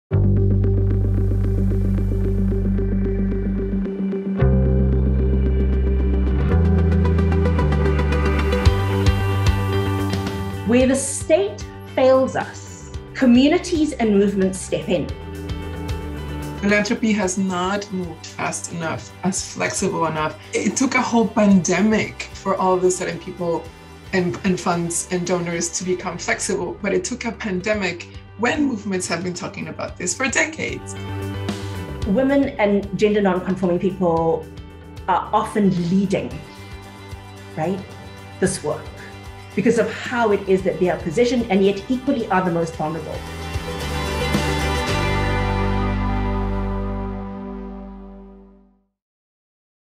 Bold Movements + Bold Funding = Bold Change (part 1)
Part 1: Take the lead from movements

During these precarious times, movements are what give us hope. Women-led movements and mobilizations have been at the forefront of resistance and sustained organizing for decades. We believe they have the greatest transformative potential to imagine and build a different future. How do we support them with the resources they need to succeed?

Inspired by our rich discussion last October with Black and brown women leaders in philanthropy, and fueled by bold intersectional feminist thinking and funding initiatives like the Black Feminist Fund, Solidaire Network, Feminist Alchemy: Building a World Beyond Crisis coordinated by the Global Fund for Women and the Collective Future Fund, amongst others, over the next few weeks we invite you to join the conversation on rethinking strategies for resourcing and supporting movements.

Here’s a soundbite 📺 of what Shereen Essof, Executive Director, JASS and Sandra Macías del Villar, Global Fund for Women have to say about meeting this movement moment collectively.